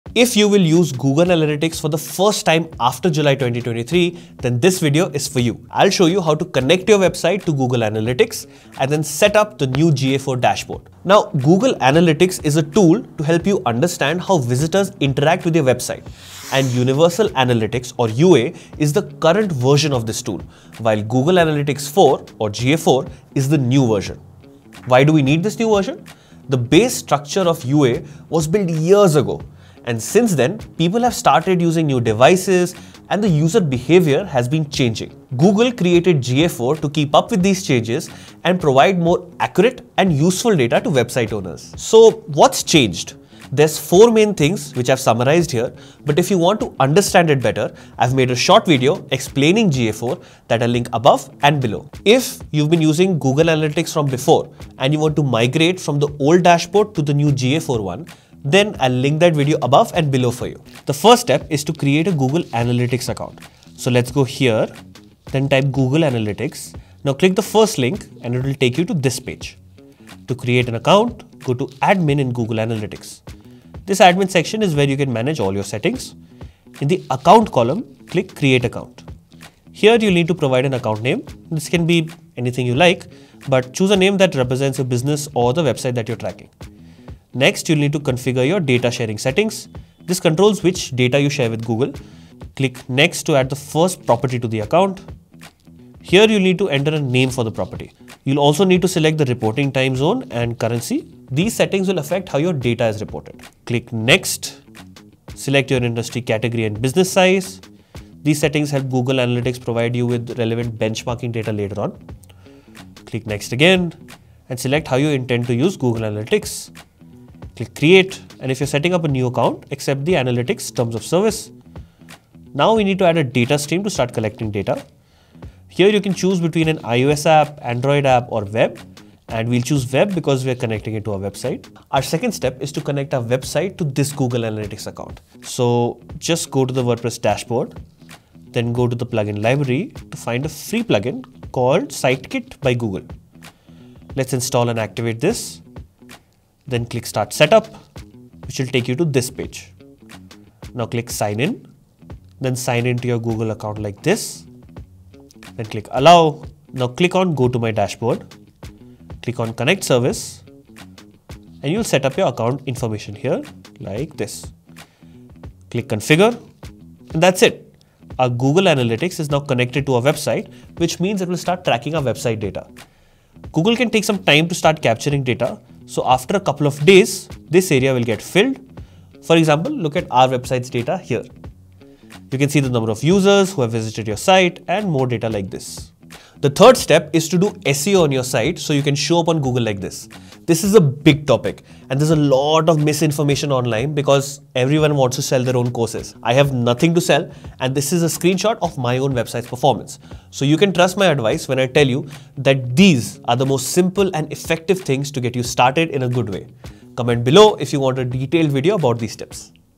2024 Beginner Tutorial - Setup GA4 (Google Analytics)
I'll help you setup the new Google Analytics 4 Dashboard. P.S. get the most from your SEO efforts with a 3 times faster site with our FREE plugin

00:00 - What is the New Google Analytics 4
01:02 - Create & Setup GA4 Account
02:15 - Connect Website to GA4 Account
03:06 - Do SEO on Website